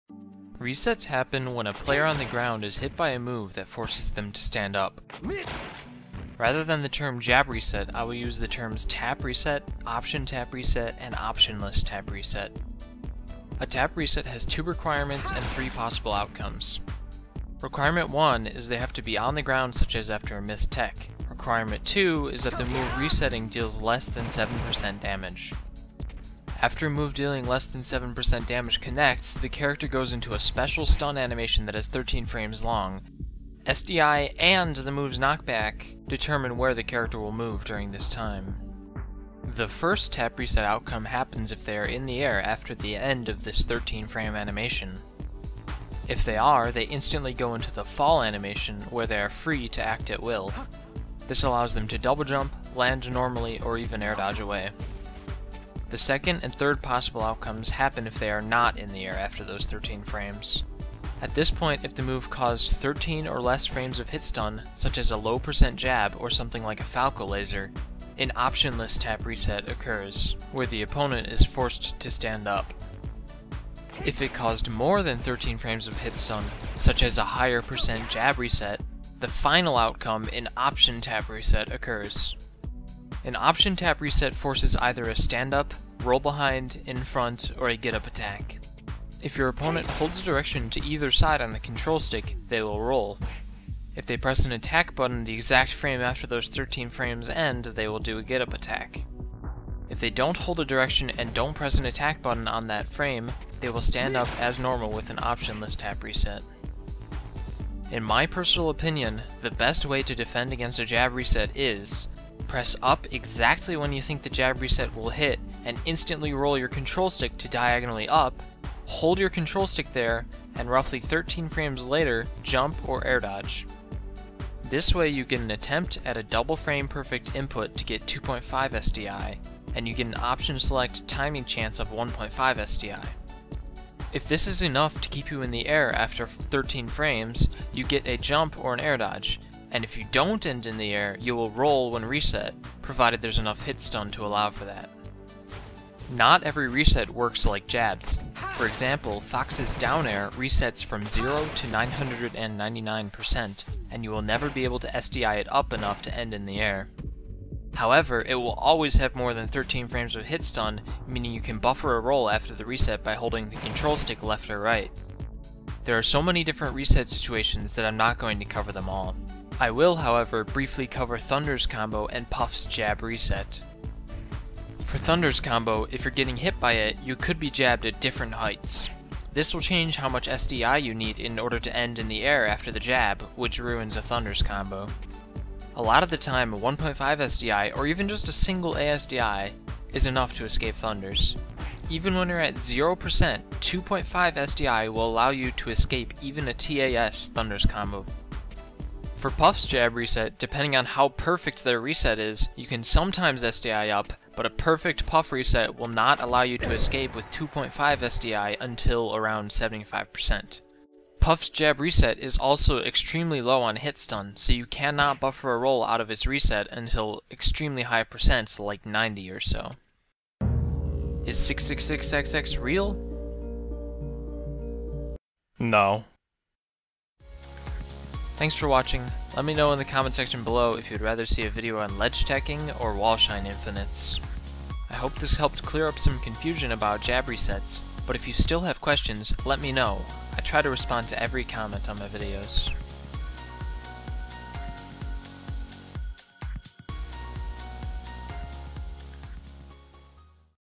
How Jab Resets Work, and How to Smash DI Them
Double jab resets function exactly the same as single jab resets, it's just a bit easier to defend against them and some character's second jabs have worse properties than the first.

SDI'ing a jab reset does not guarantee that you are safe. There are still options for the opponent, such as a second jab, a read, etc.

Hopefully with this video you'll understand how to defend against jab resets much better than you did before.